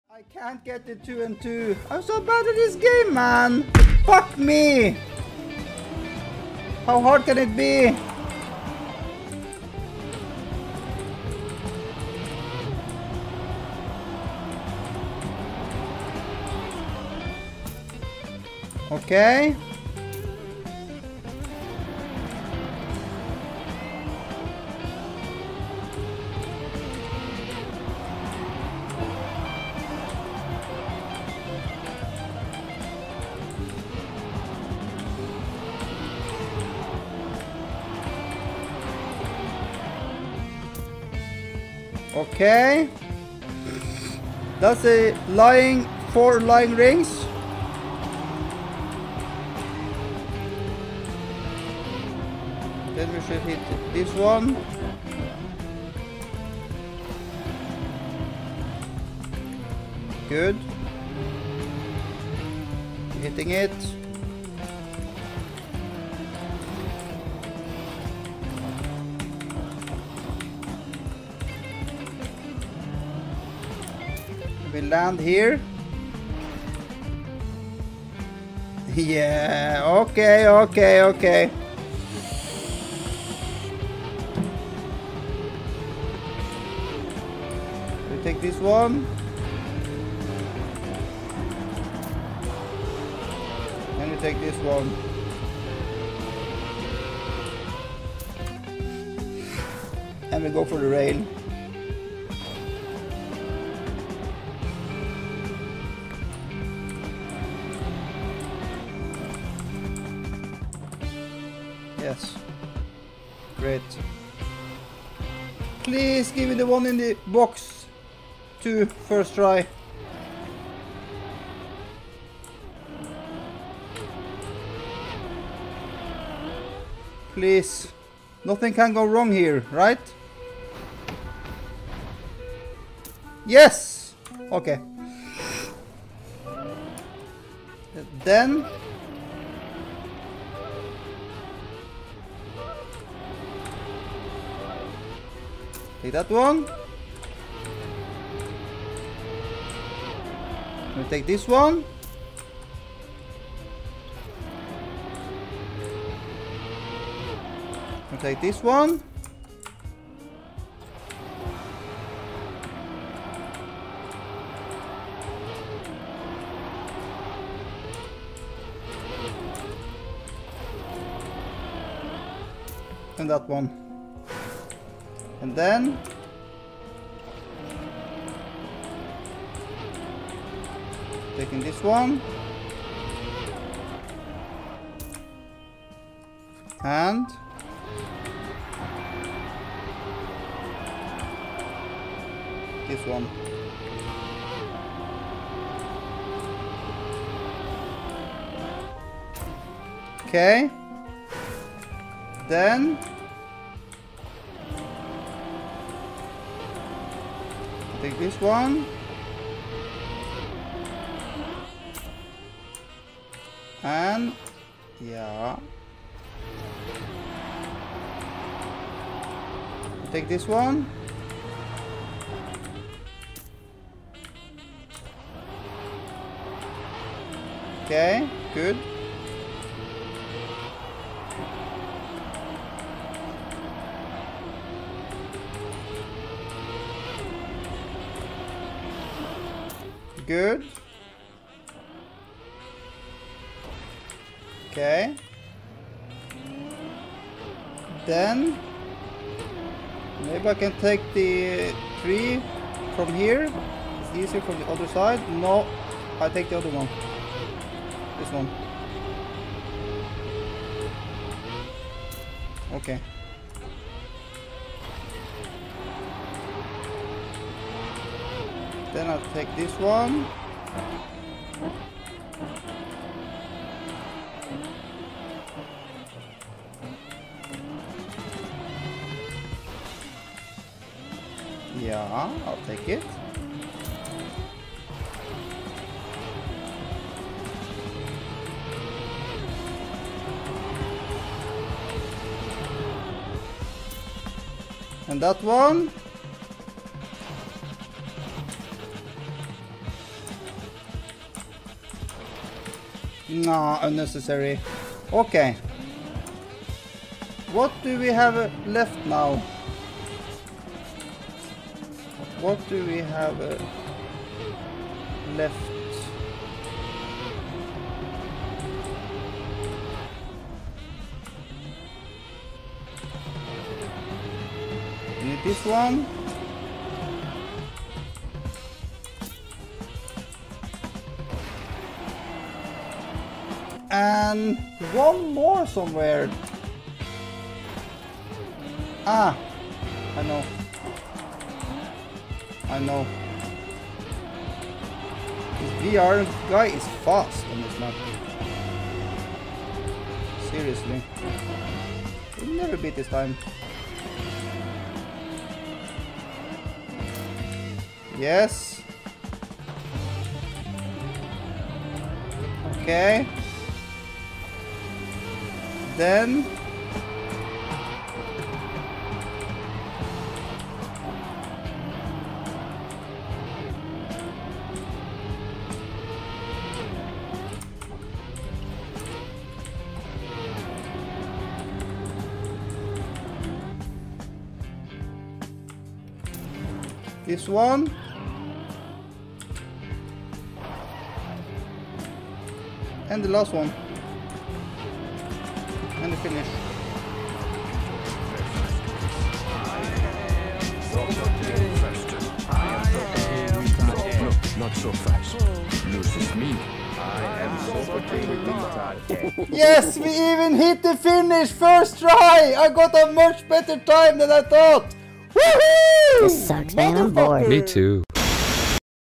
Genius mapper made my dreammap into full dirt!
Well done mapper - very fun!

Original full ice by Tag666, here!